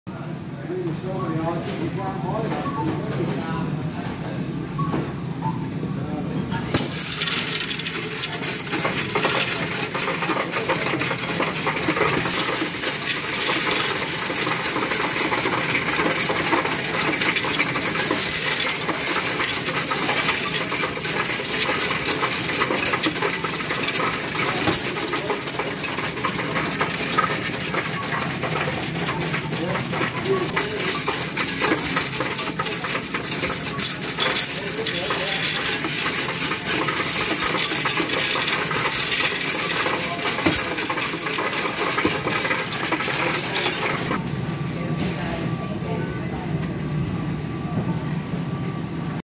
Coinstar at Tescos
The banks are very reluctant to take your change now, found one of these machines the only drawback is they take a percentage of your cash at the final total.

Loved using it ,reminds me of the 2p machines in the arcades.